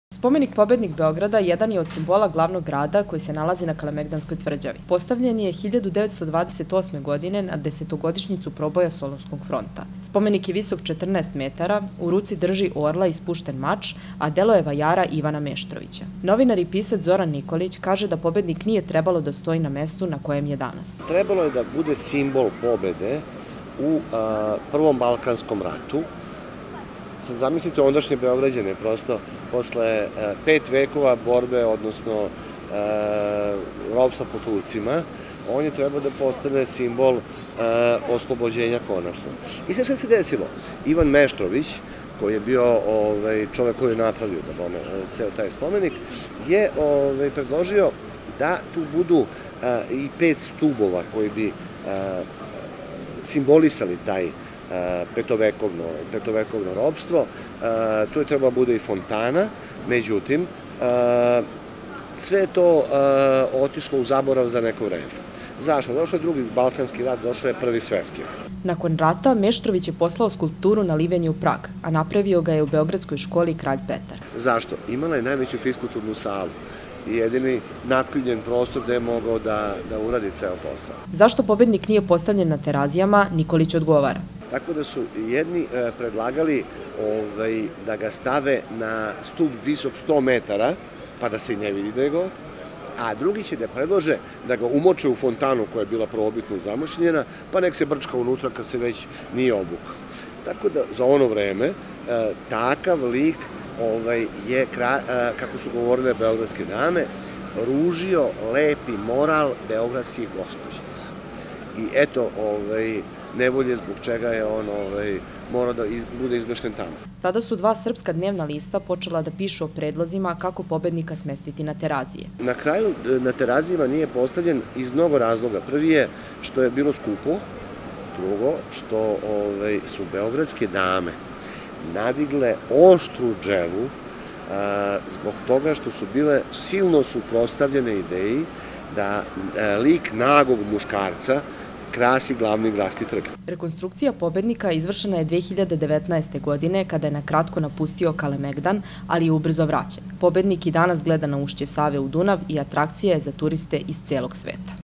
Spomenik Pobednik na Kalemegdanskoj tvrdjavi u Begradu
Spomenik Pobednik Beograda, simbol je glavnog grada. Postavljen je 1928. i nalazi se na Kalemegdanskoj tvrdjavi.  Medjutim, novinar i pisac Zoran Nikolić kaže da Pobednik nije trebalo da stoji na mestu na kojem je danas.Prilog Kineske medijske grupe (CMG)